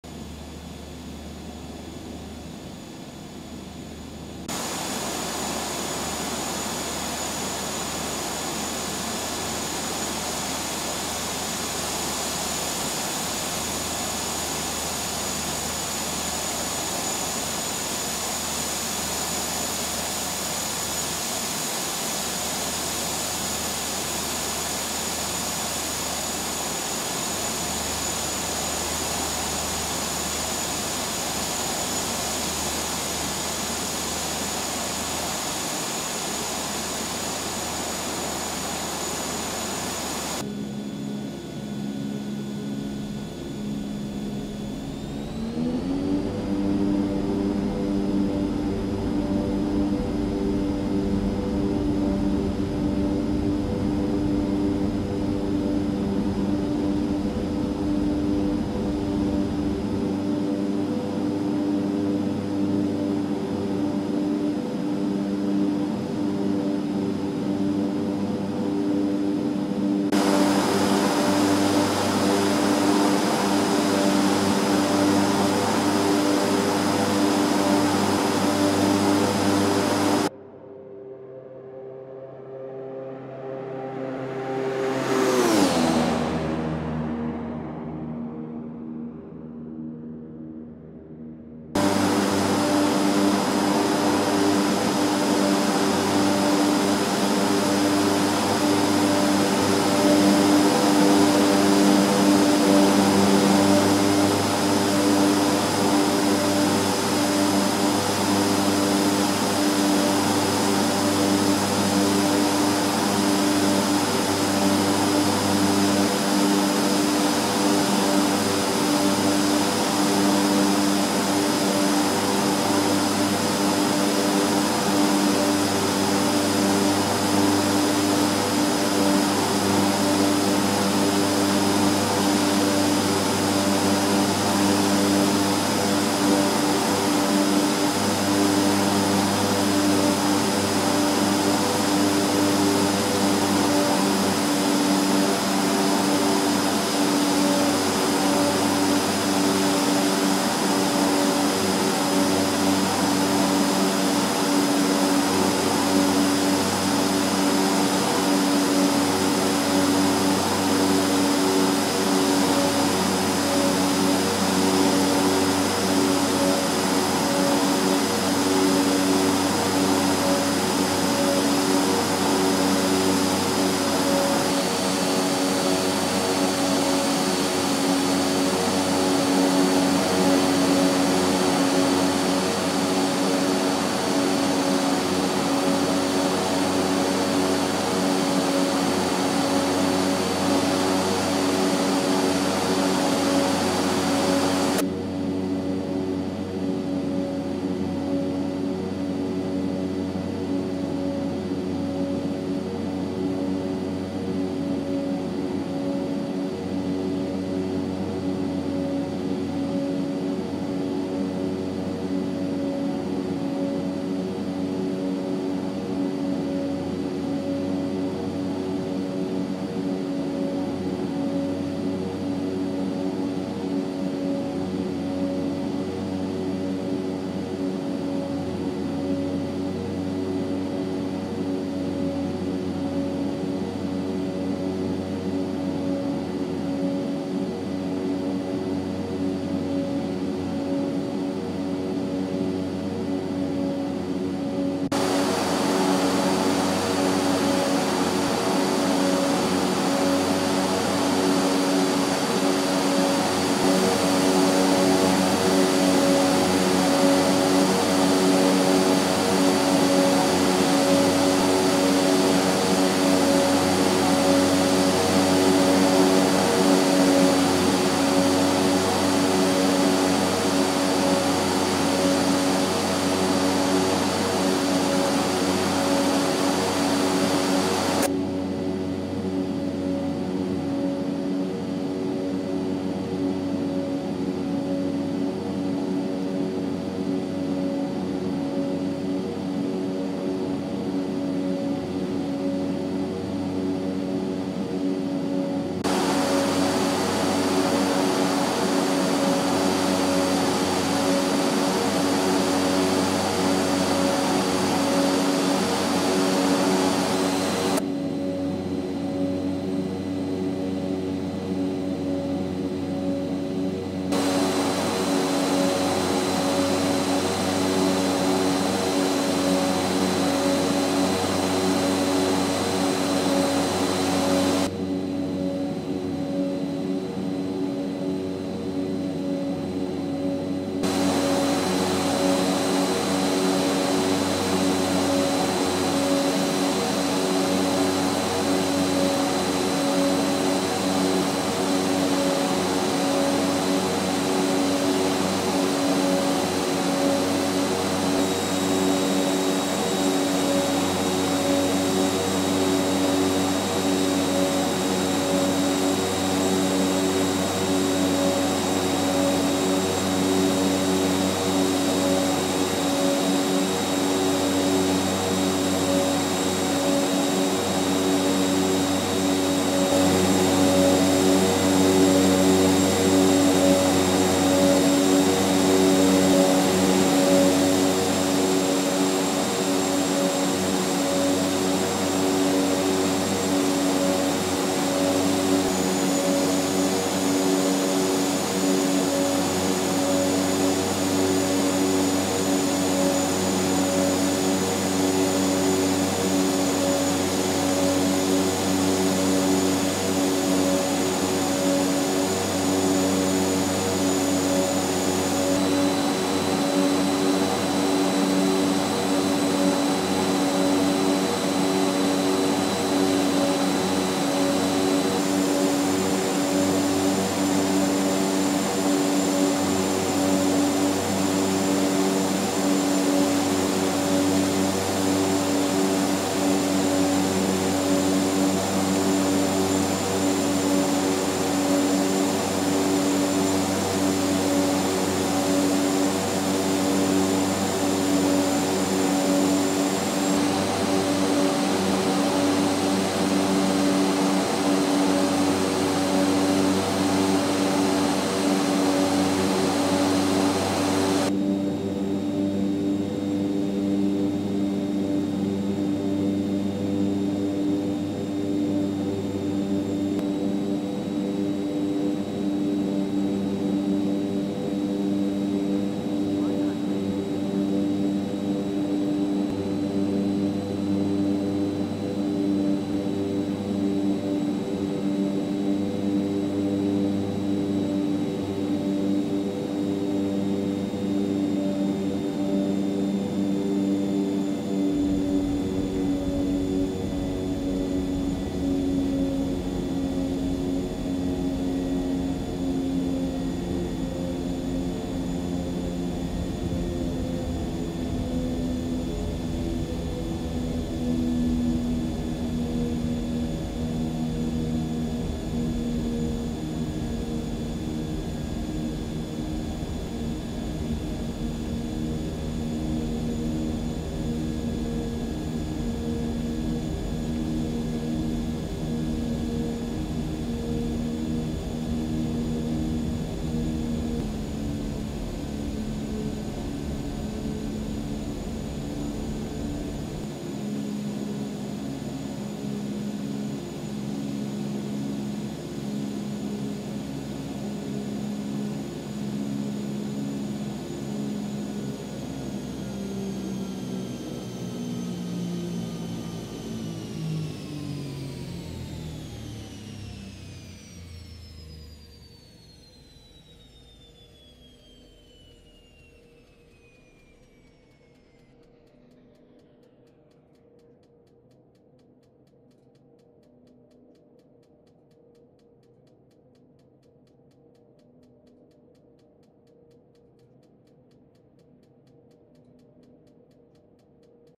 Epic E1000 sim flight Aswan to Abu Simbel
The Epic E1000 is an incredible turboprop with amazing specs. For this sim flight, I departed from Aswan, Egypt, and landed in Abu Simbel near Egypt's southern border with Sudan. I used real time and weather.